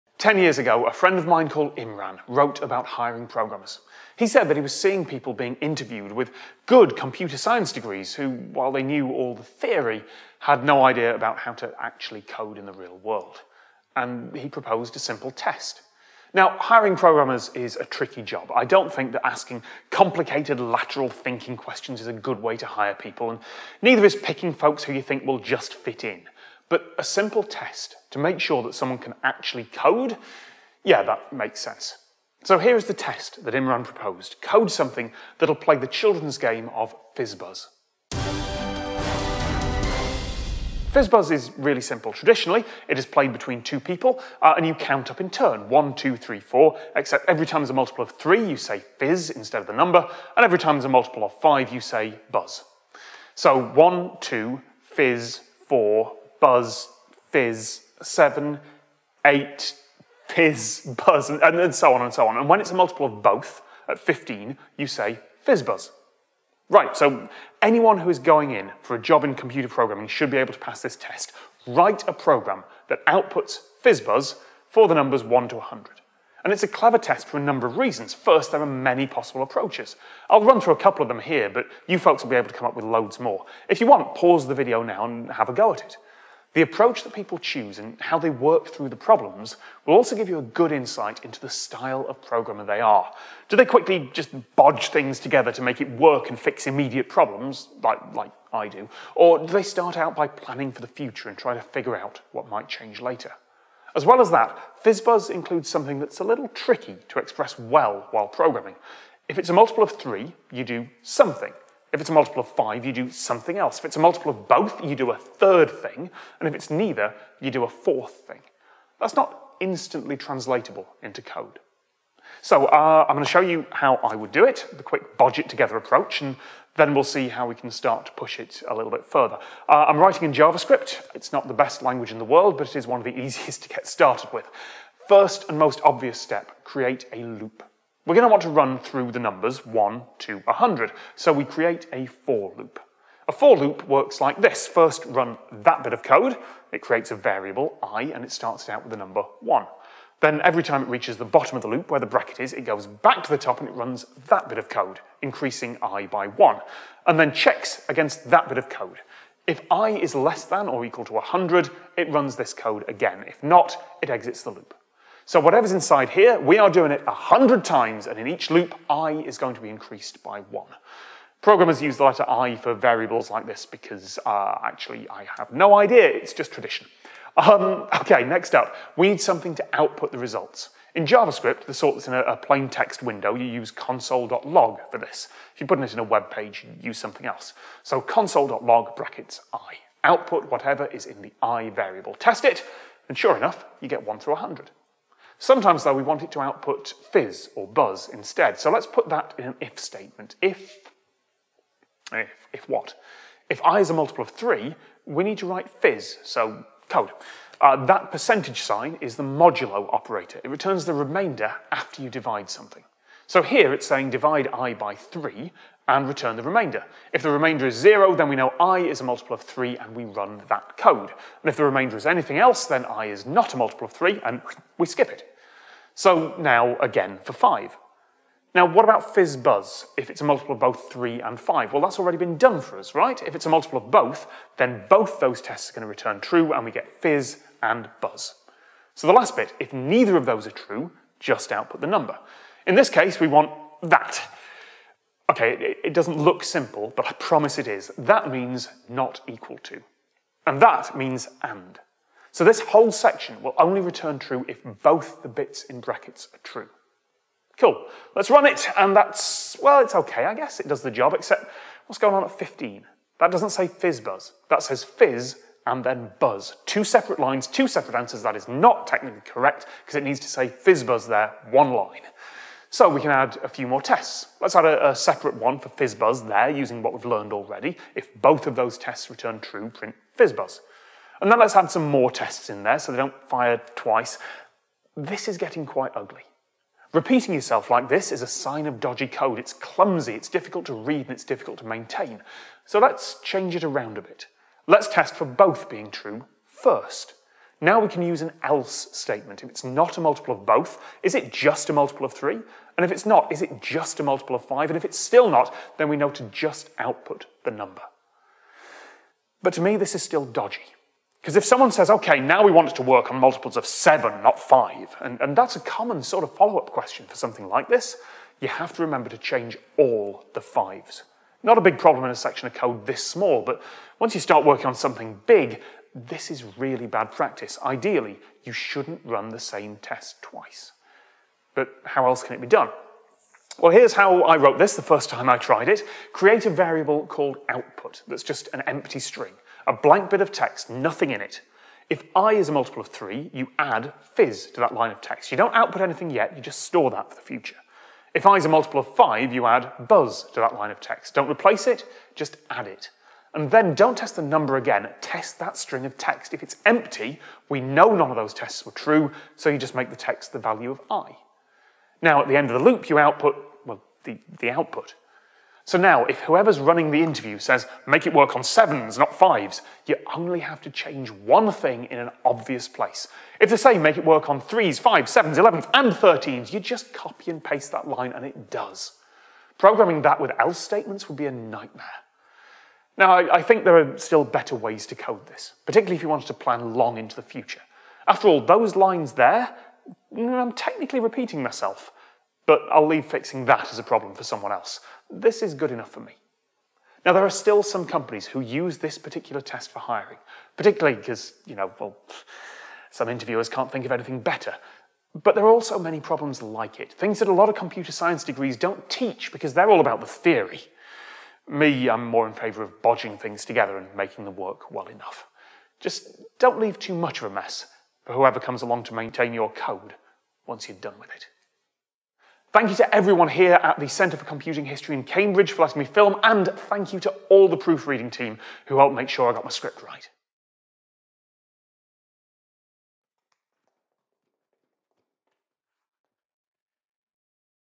FizzBuzz: One Simple Interview Question
There are a lot of opinions on how to hire coders, and most of them are terrible. The opinions, that is, not the coders. But a basic filter test to make sure someone can do what they say they can: that seems reasonable, and FizzBuzz is one of the more common tests. Even now, interviewers use it. Let's talk about why it's tricky, and how to solve it.

And thanks to my proofreading team!